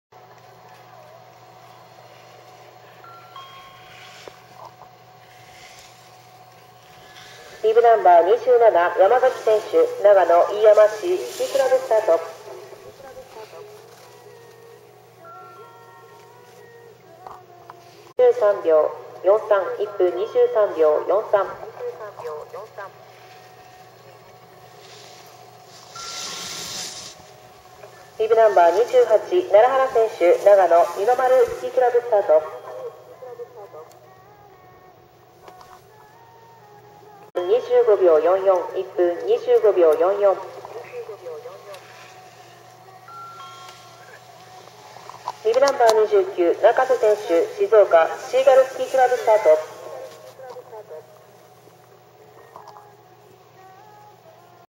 110308ほうのき平1018～男子75歳代
第14回ほうのき平マスターズアルペン大会の1本目
男子75歳～79歳
私の父親世代です。恐れ入りましたm(..)m